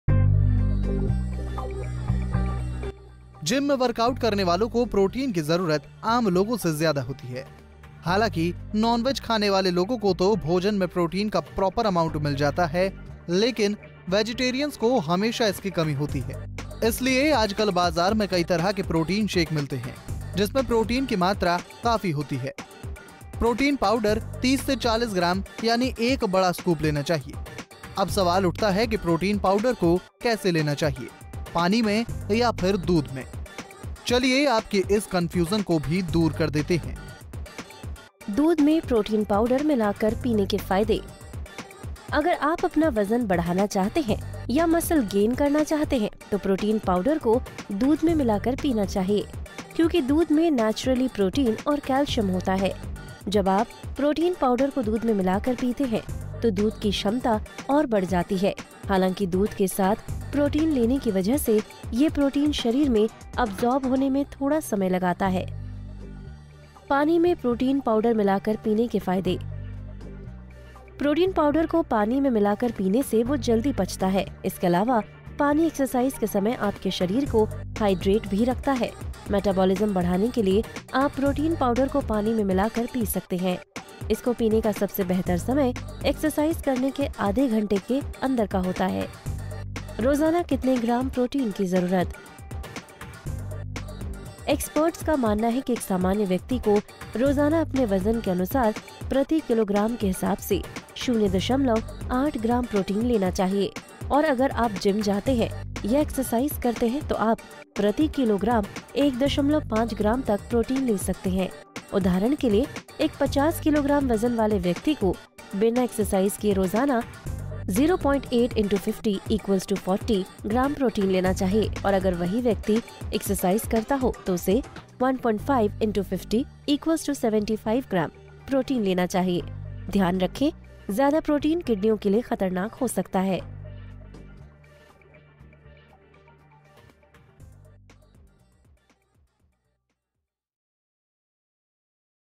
How to take protein? with Milk or Water? | Hindi
in this video, your doubt about how to take protein in water or milk will be clear. Even you come to know about many other things, like how much protein you have to take in a day and much more.